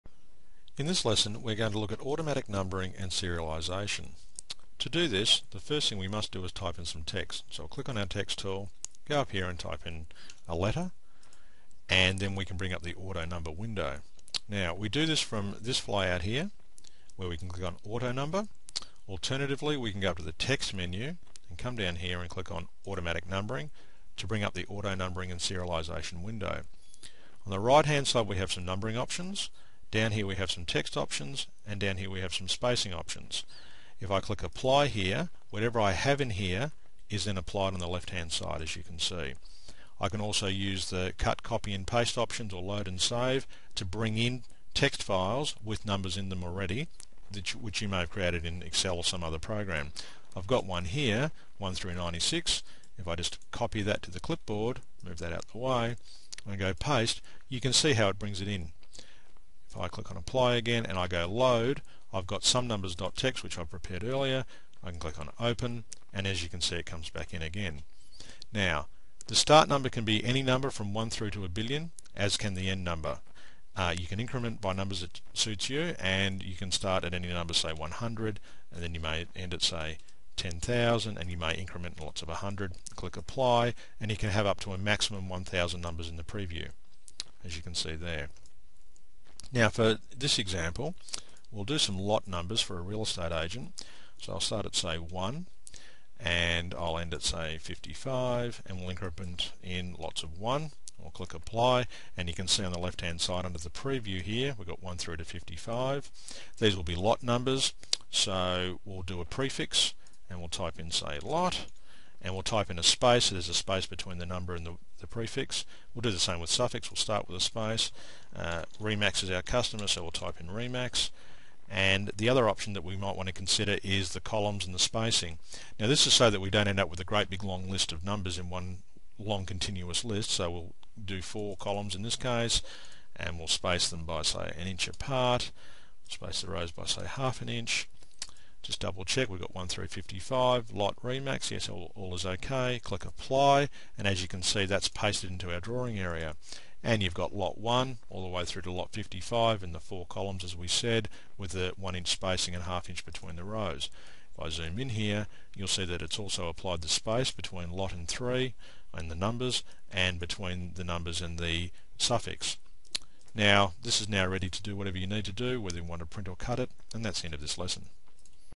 Working with in Auto Numbering (Serialization) VinylMaster
Where and how to work with Auto Numbering (Serialization) in VinylMaster